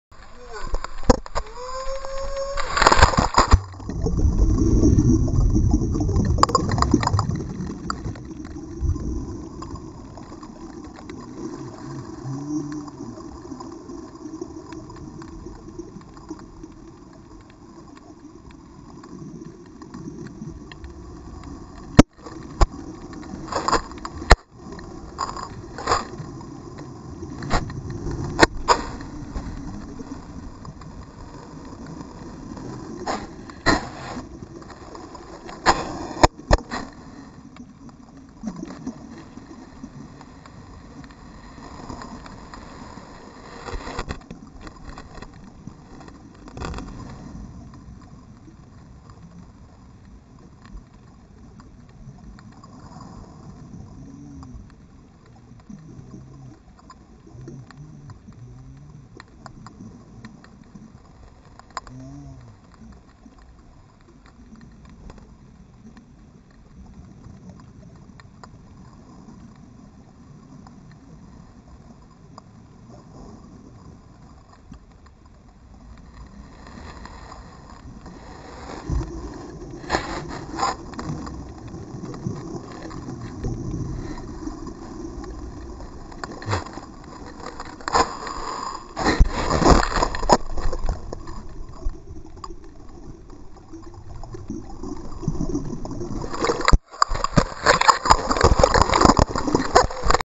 HOW TO LOSE A GOPRO CAMERA.mp4
always attach a leash to your gopro camera!
fortunately for me i covered mine in fluro tape and it got some awesome shots on its own whilst rolling on the bottom of the sea.